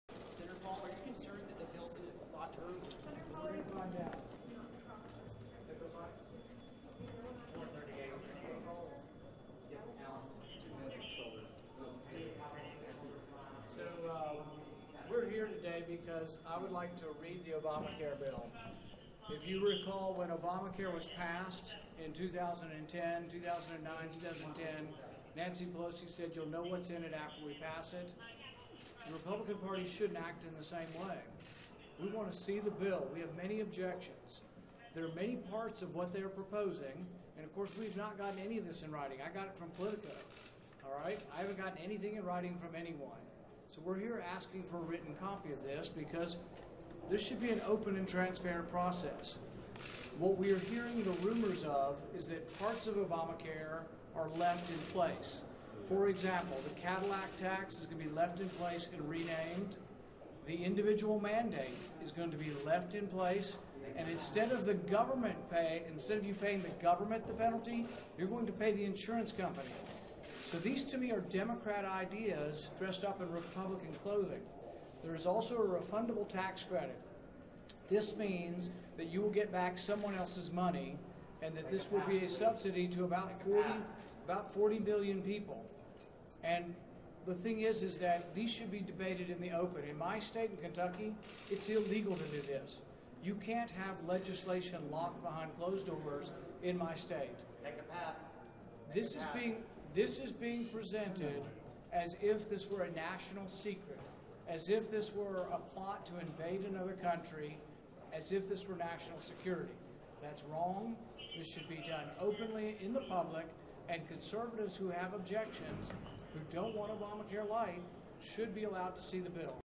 Paul Ryan and Mitch McConnell REFUSE to let Sen Rand Paul See the New ObamaCare Replacement Bill
Sen. Rand Paul (R) marched to the House side of the Capitol, knocked on a locked door and demanded to see a copy of the House's bill to repeal and replace the Affordable Care Act, which he believed was being kept under lock and key.